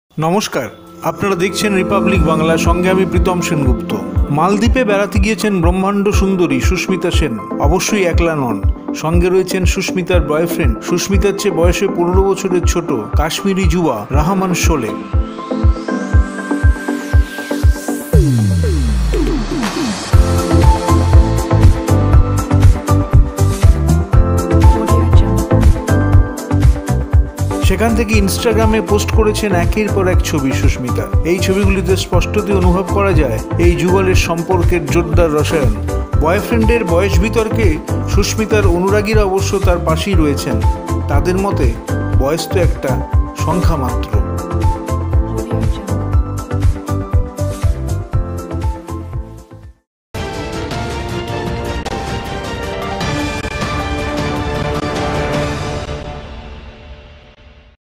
মালদ্বীপে বয়ফ্রেন্ডের সঙ্গে সুস্মিতা
মালদ্বীপে বেড়াতে গিয়েছেন ব্রহ্মান্ড সুন্দরী সুস্মিতা সেন। অবশ্যই একলা নন , সঙ্গে রয়েছেন সুস্মিতার বয় ফ্রেন্ড , সুস্মিতার চেয়ে বয়সে ১৫ বছরের ছোট , কাশ্মীরি যুবা রহমান শোলে। সেখান থেকে ইনস্টাগ্রামে পোস্ট করেছেন একের পর এক ছবি।